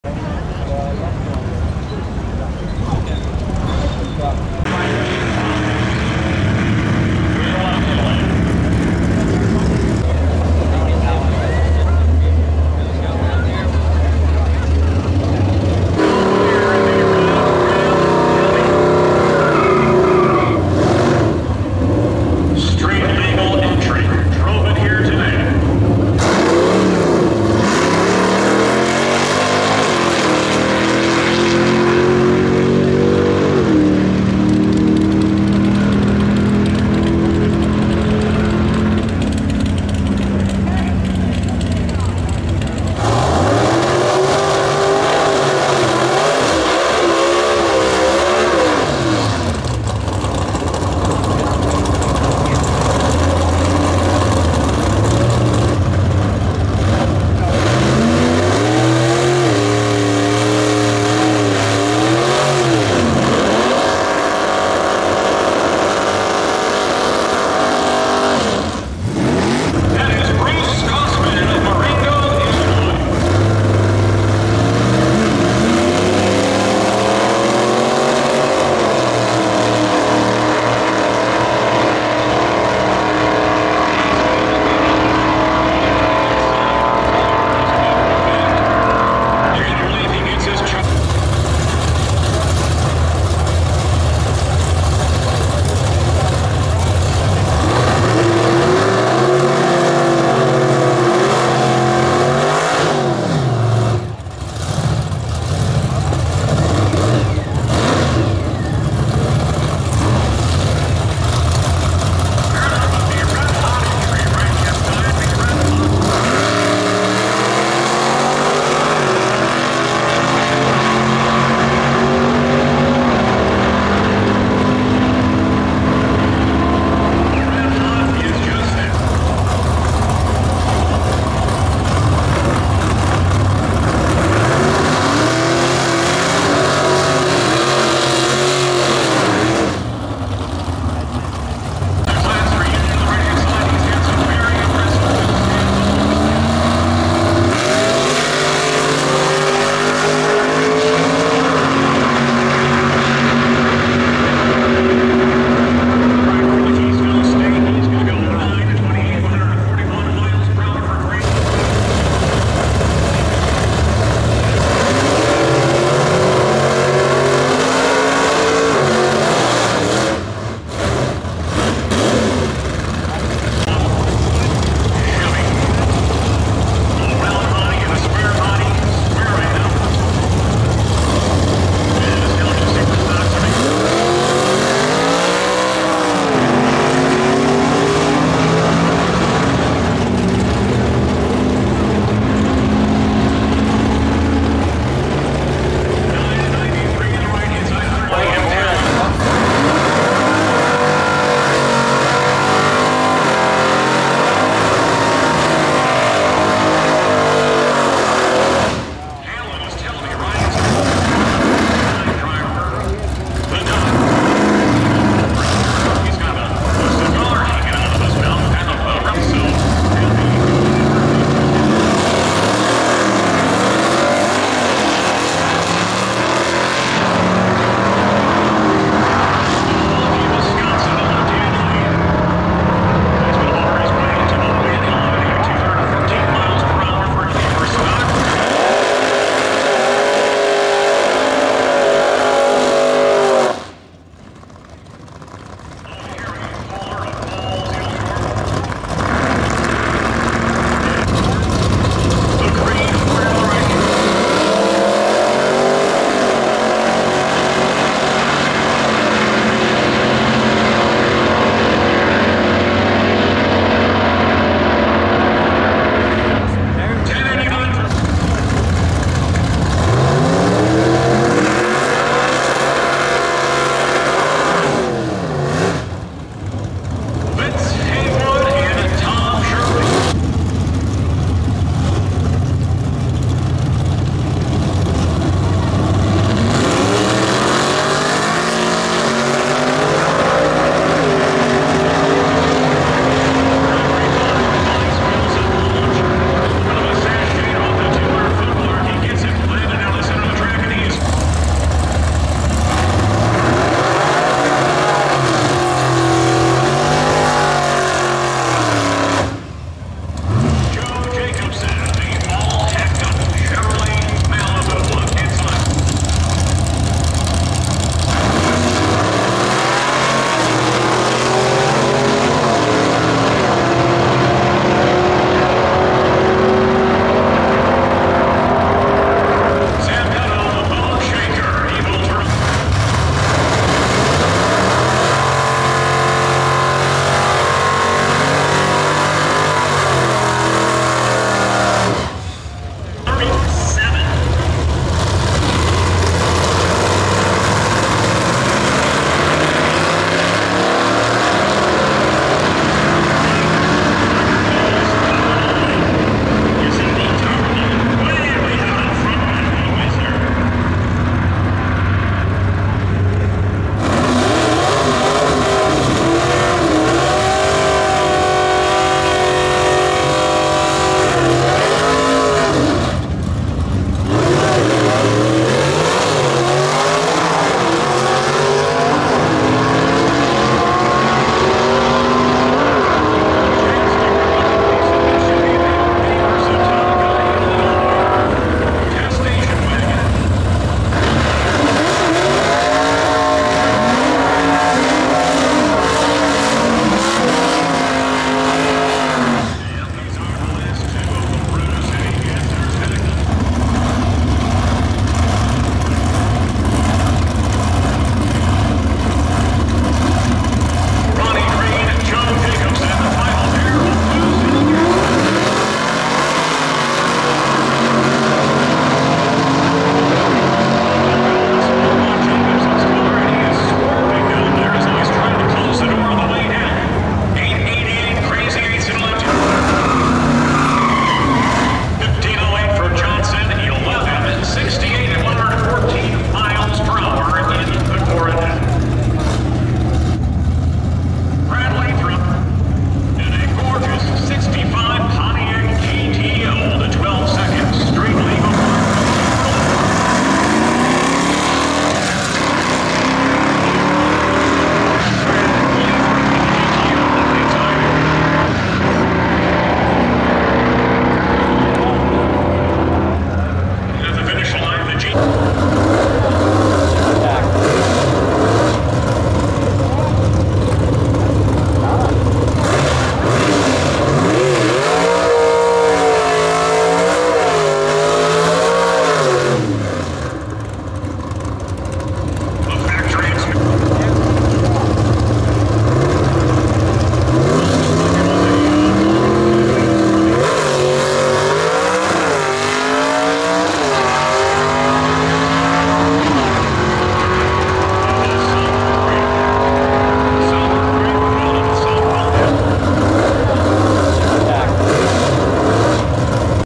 Glory Days are Back Again Cars of The 60s and Earlier Vintage Drag Race at Byron Dragway
Glory Days